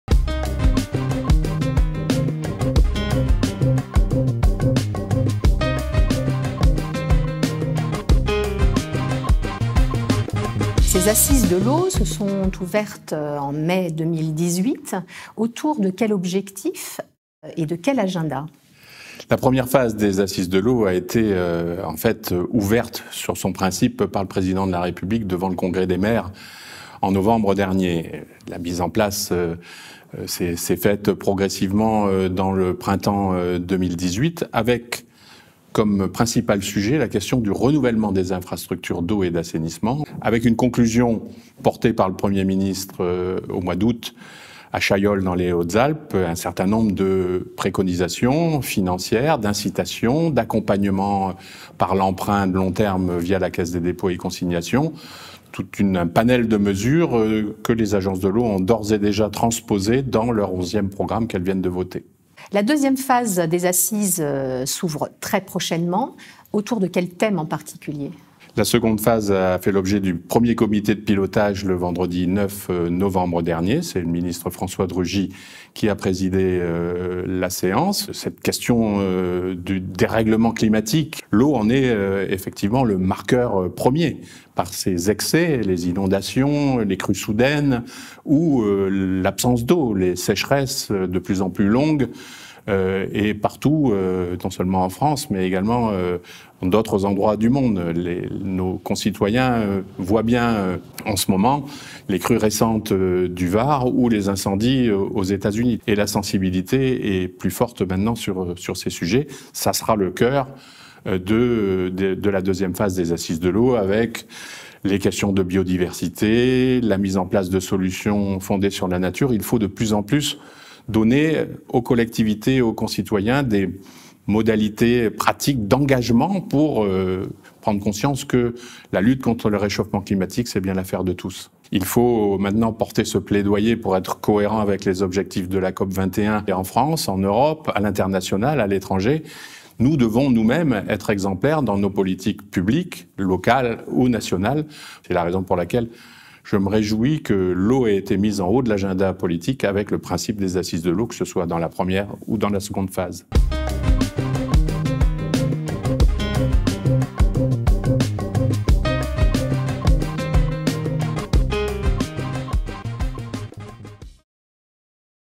"L'eau est le marqueur premier du dérèglement climatique" - Jean Launay
Jean Launay,  coordinateur des Assises de l'Eau, Président du Comité National de l'Eau et Président du Partenariat Français pour l'Eau, nous parle des Assises de l'Eau et des problématiques nouvelles qui se présentent au monde de l'eau.